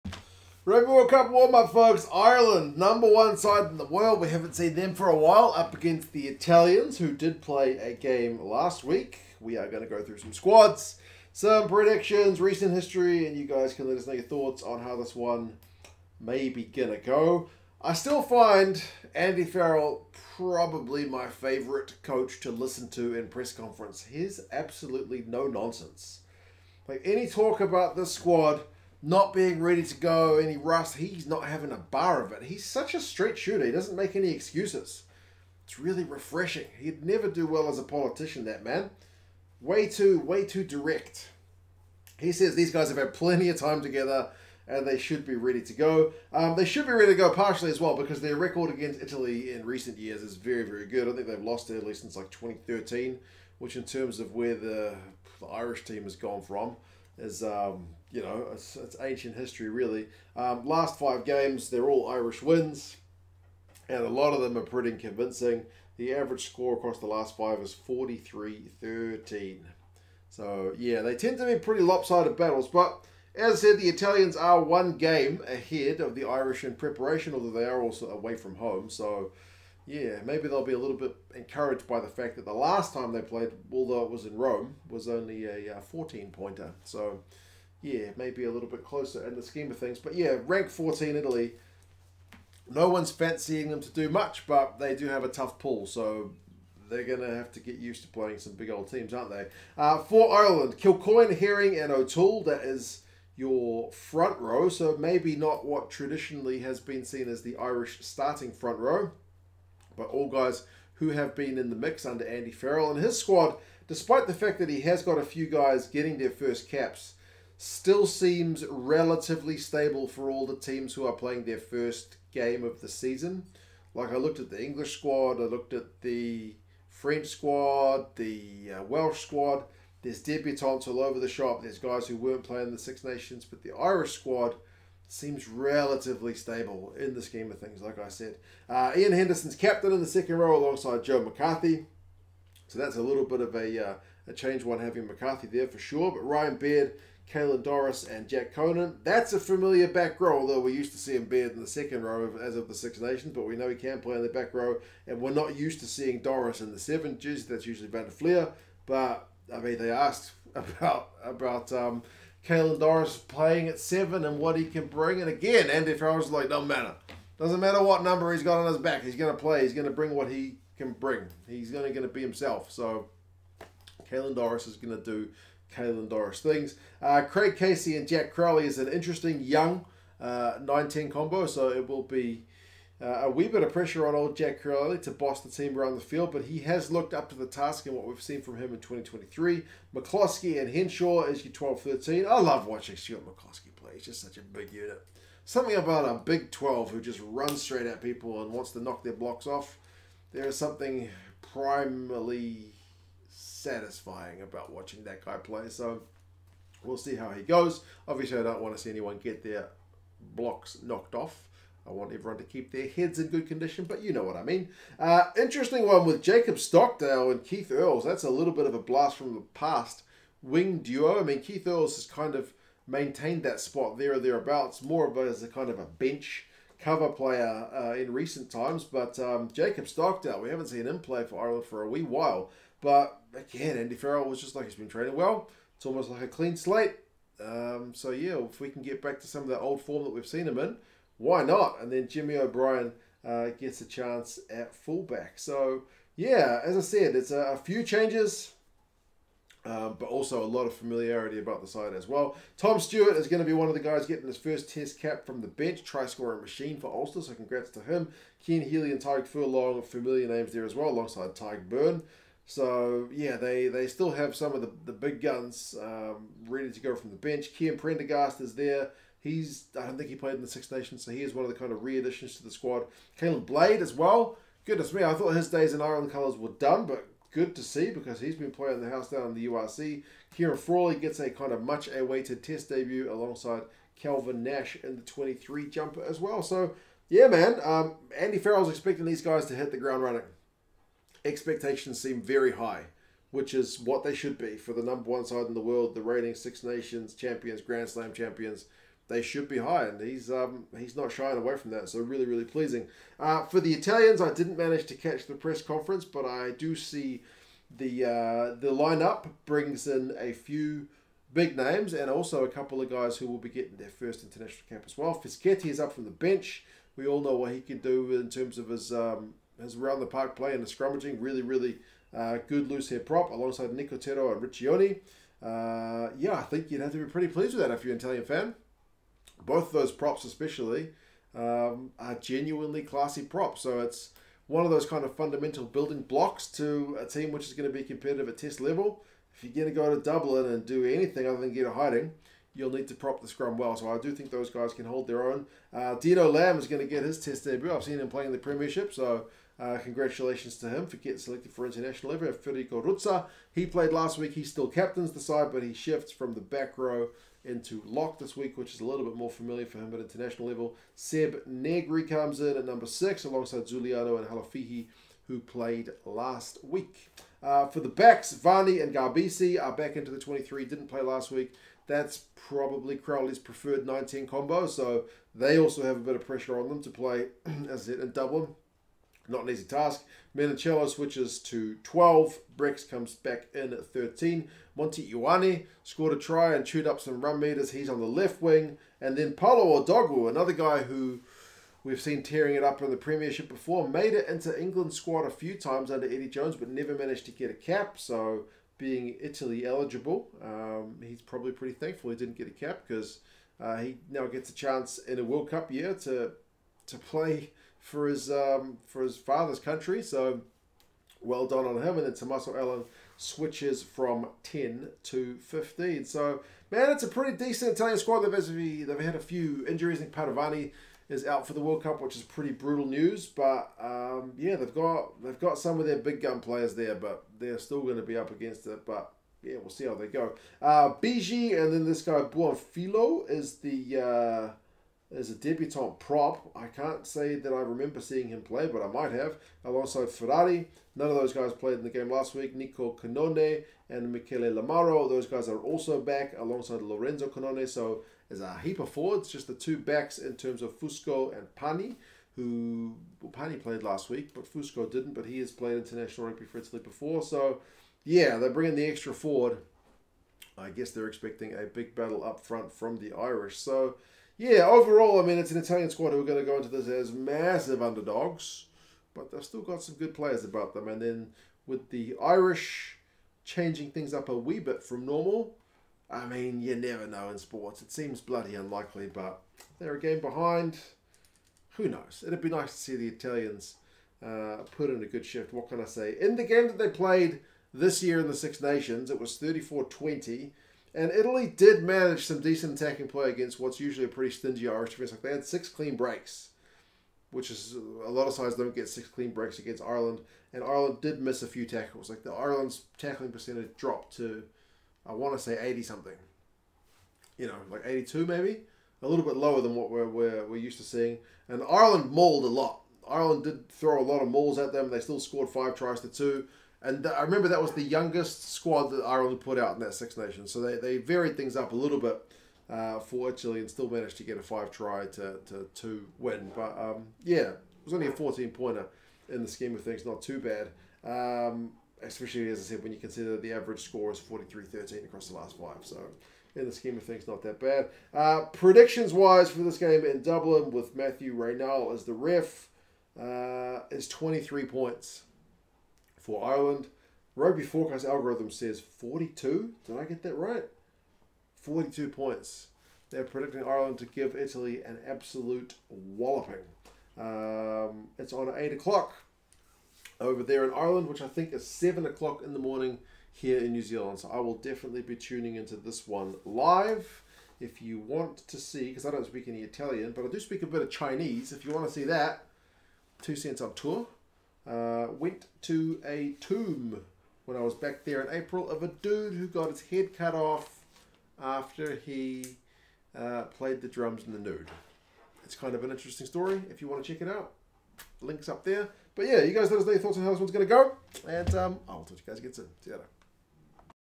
Ireland v Italy Preview - Rugby World Cup 2023 Warm Up
Ireland vs Italy Preview, Summer Nations Series: Lineups, Recent History & Predictions

Ireland: 15 Jimmy O’Brien, 14 Keith Earls, 13 Robbie Henshaw, 12 Stuart McCloskey, 11 Jacob Stockdale, 10 Jack Crowley, 9 Craig Casey, 8 Jack Conan, 7 Caelan Doris, 6 Ryan Baird, 5 Joe McCarthy, 4 Iain Henderson (c), 3 Tom O’Toole, 2 Rob Herring, 1 David Kilcoyne
Replacements: 16 Tom Stewart*, 17 Cian Healy, 18 Tadhg Furlong, 19 Tadhg Beirne, 20 Cian Prendergast, 21 Caolin Blade, 22 Ciaran Frawley*, 23 Calvin Nash*

Italy: 15 Tommaso Allan; 14 Paolo Odogwu*, 13 Juan Ignacio Brex, 12 Tommaso Menoncello, 11 Monty Ioane; 10 Paolo Garbisi, 9 Stephen Varney; 1 Danilo Fischetti, 2 Giacomo Nicotera, 3 Marco Riccioni; 4 Dino Lamb*, 5 Federico Ruzza (c); 6 Sebastian Negri, 7 Manuel Zuliani, 8 Toa Halafihi.
Replacements: Luca Bigi, Paolo Buonfiglio*, Simone Ferrari, Niccolo Cannone, Michele Lamaro, Lorenzo Cannone, Alessandro Fusco, Lorenzo Pani.

VPN: Express VPN - 3 Months Free